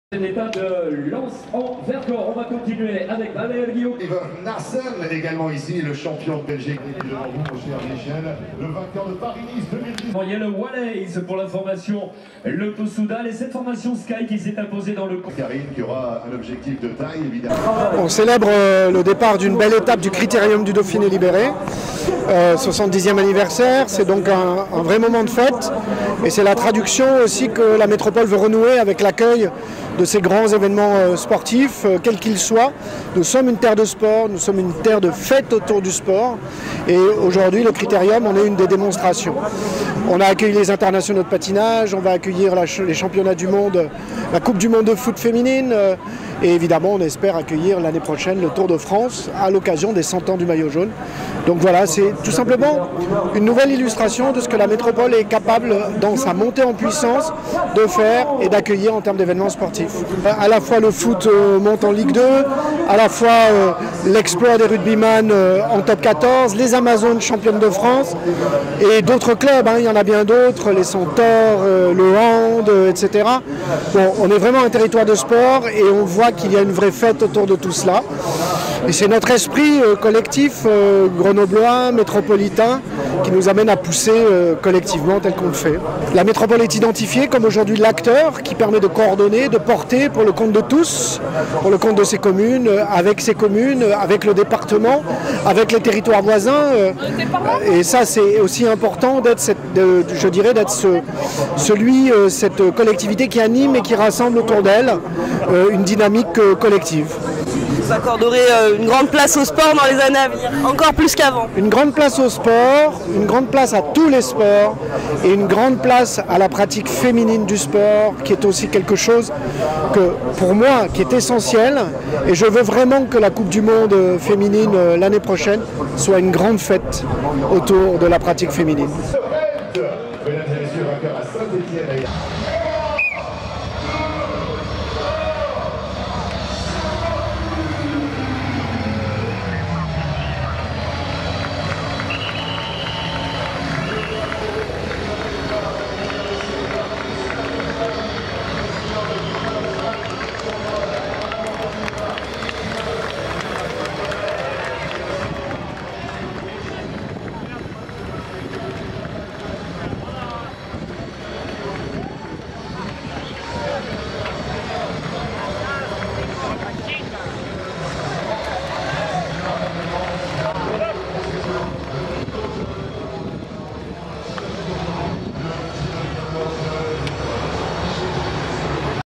Critérium du Dauphiné 2018 : cinquième étape au départ de Grenoble
Départ de la cinquième étape du Critérium Du Dauphiné 2018 ce midi de Grenoble, pour parcourir 130km jusqu'à Valmorel. L'occasion de célébrer la 70ème édition du Critérium au départ de l’étape.